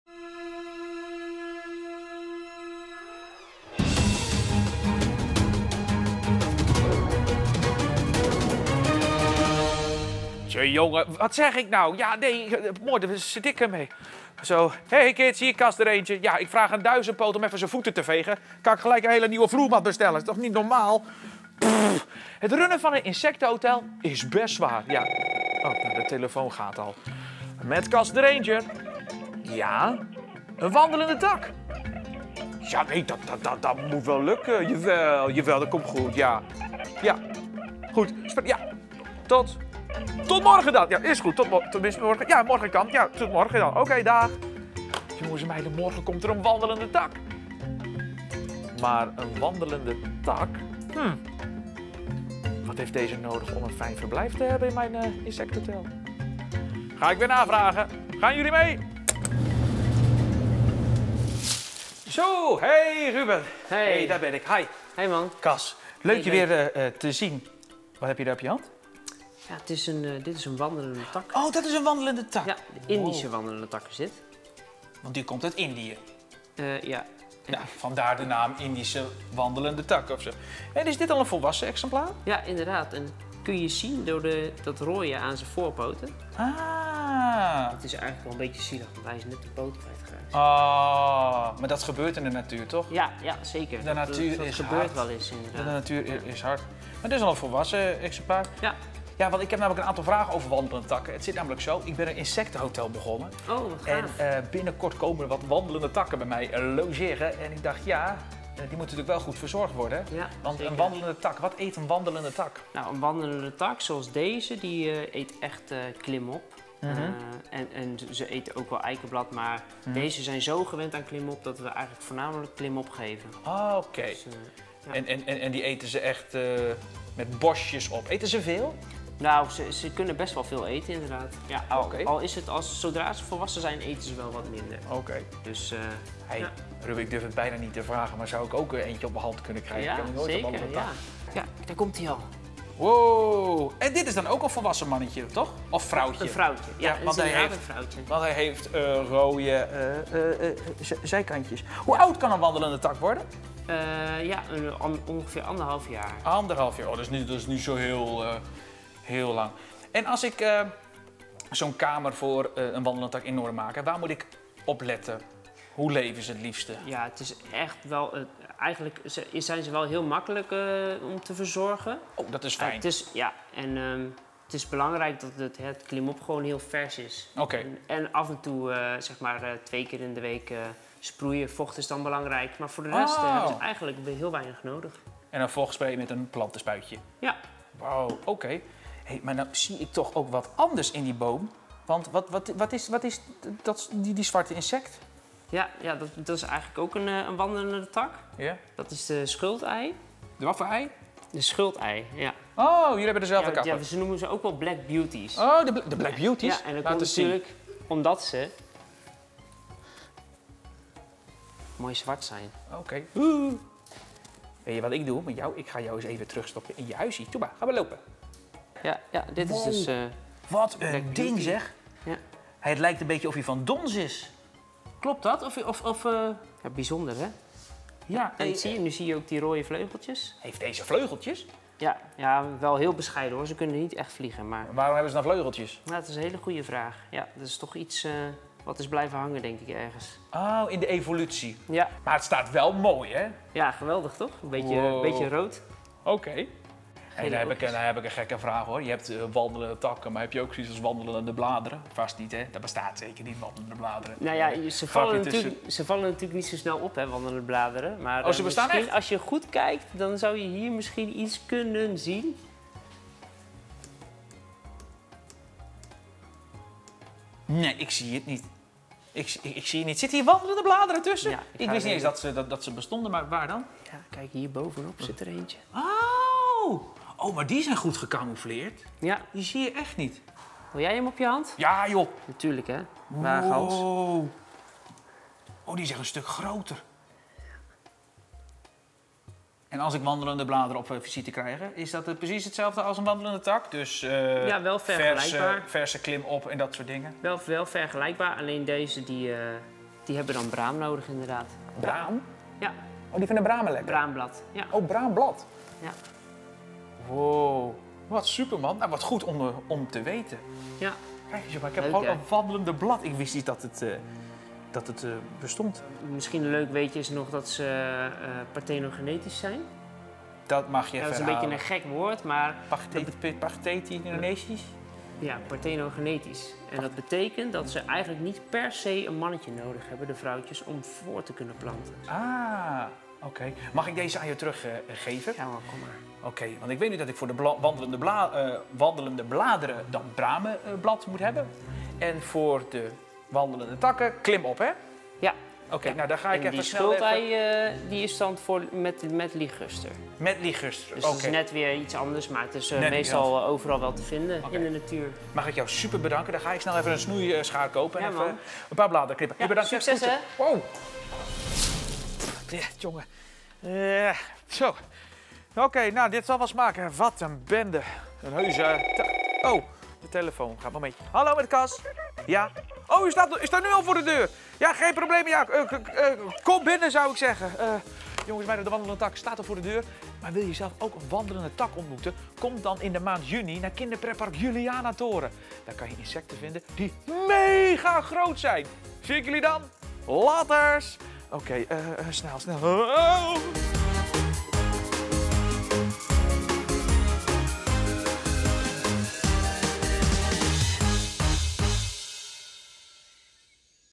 CAS ZIJN INSECTENHOTEL - Wandelendetakken 🍃
Cas de Ranger begint zijn eigen insectenhotel. Alle insecten kunnen komen logeren in zijn Insectenhotel. Maar wat heeft Cas allemaal nodig zodat de insecten een fijn verblijf hebben in zijn insectenhotel? Je ziet het in deze video.

Volg Kinderpretpark Julianatoren ook op onze social media kanalen en blijf op de hoogte van alle te gekke nieuwtjes!

 Julianatoren, te gek!